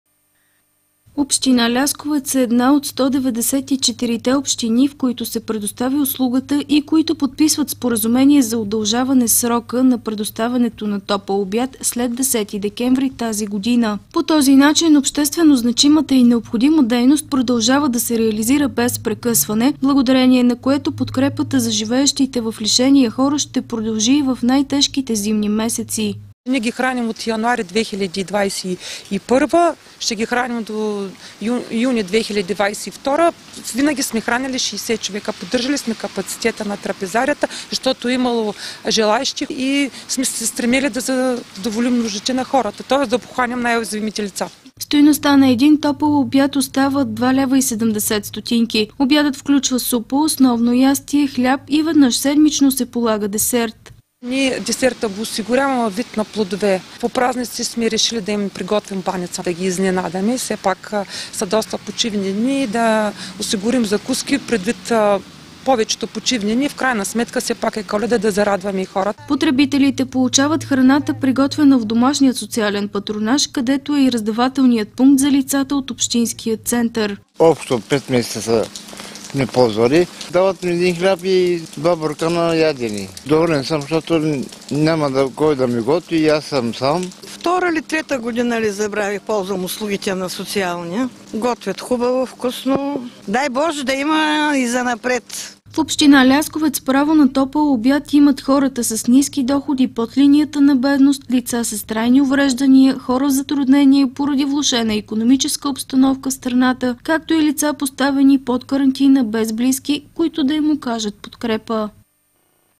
Срокът за получаване на топъл обяд за нуждаещите се граждани от Община Лясковец се удължава
На топъл обяд в продължение на още половин година ще имат право 60 лица от Община Лясковец. Потребителите на Обществената трапезария ще продължат да се хранят безплатно до 30 юни 2022 година. Това стана възможно, след решение на служебното правителство. Ползвателите на социалната услуга в Общината са от града и от селата Джулюница и Добри дял и Козаревец.
Община Лясковец е една от 194-те общини, в които се предоставя услугата и които подписват споразумение за удължаване срока на предоставянето на топъл обяд след 10 декември тази година. По този начин обществено значимата и необходима дейност продължава да се реализира без прекъсване, благодарение на което подкрепата за живеещите в лишения хора, ще продължи и в най-тежките зимни месеци.
 Ваня Табакова, управител на Домашен социален патронаж – Лясковец:
 “Ние ги храним от януари 2021, ще ги храним до юни 2022 година. Винаги сме хранили 60 човека, поддържали сме капацитета на трапезарията, защото е имало желаещи и сме се стремили да задоволим нуждите на хората, тоест да обхванем най-уязвимите лица.”
Стойността на един топъл обяд остава 2.70 лева. Обядът включва супа, основно ястие, хляб и веднъж седмично се полага десерт.
  „Ние десерта го осигуряваме във вид на плодове. По празниците сме решили да им приготвим баница, да ги изненадаме. Все пак са доста почвни дни и да осигурим закуски, предвид повечето почивни дни. В крайна сметка все пак е Коледа, да зарадваме хората.“
Потребителите получават храната, приготвена в Домашния социален патронаж, където е и раздавателният пункт за лицата от общинския център.
 Амет:
  “Общо пет месеца сме ползвали.  Дават ми един хляб и два буркана ядене.  Доволен съм, защото няма кой да ми готви, аз съм сам.”
 „Втора или трета година ползвам услугите на социалния. Готвят хубаво, вкусно.
  Дай Боже да има и занапред.“
В община Лясковец право на топъл обяд имат хората с ниски доходи под линията на бедност, лица с трайни увреждания, хора в затруднение поради влошената икономическа обстановка в страната, както и лица поставени под карантина без близки, които да им окажат подкрепа.